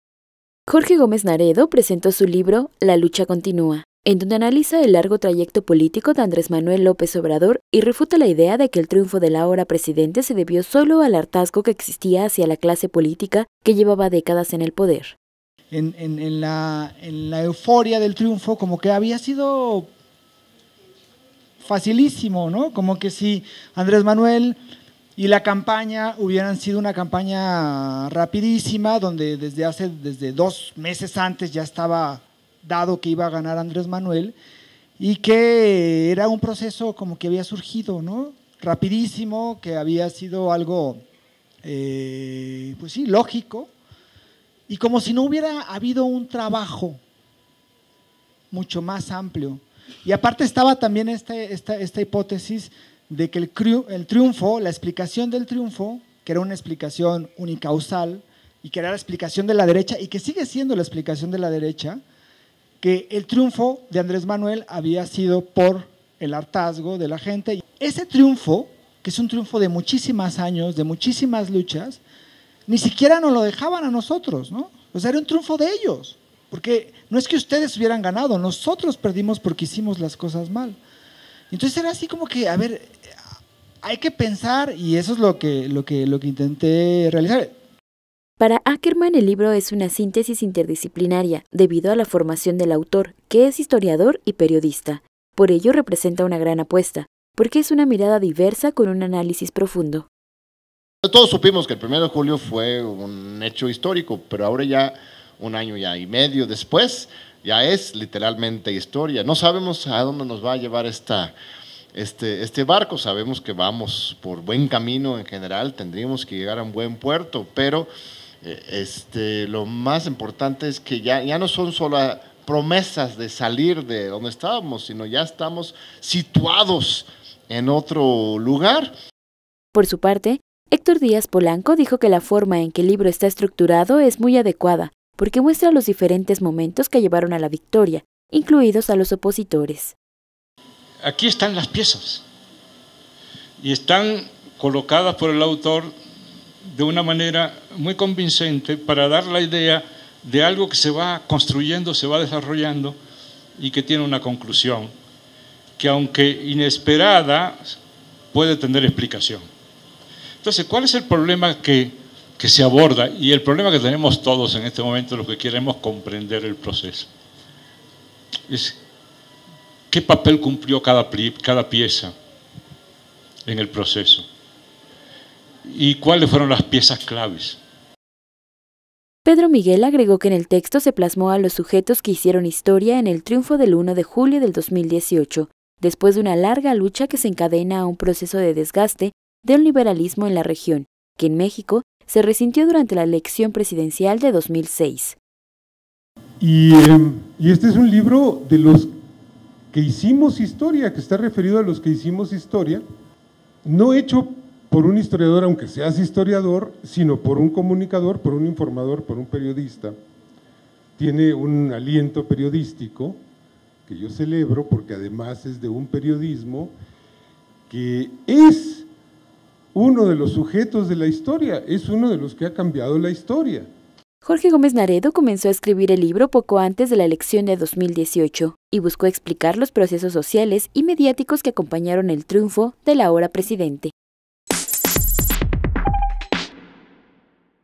“La lucha continúa” refuta hipótesis que regateaba triunfo de AMLO
Jorge Gómez Naredo comenzó a escribir el libro poco antes de la elección de 2018 y buscó explicar los procesos sociales y mediáticos que acompañaron el triunfo del ahora presidente.